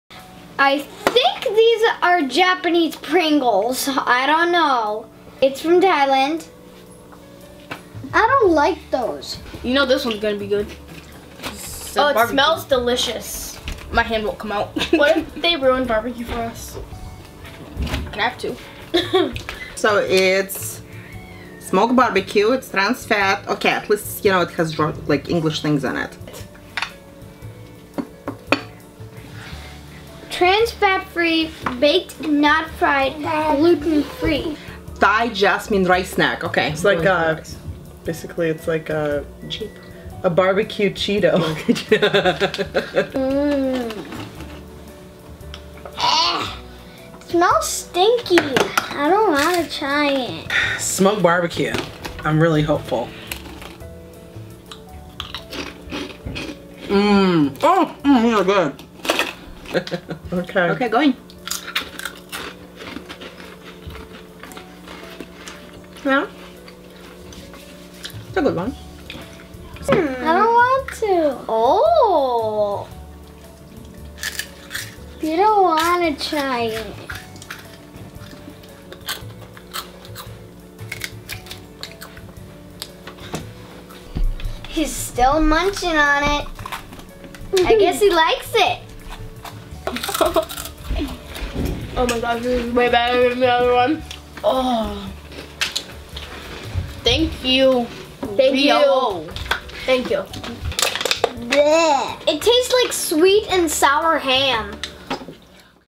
WE ARE TRYING THAI PRINGLES | THAI SNACKS
Do Pringles taste the same in Thailand. Get ready for the explosion of taste bud - we are trying another Thai Snack and this might not be as delicious as some might think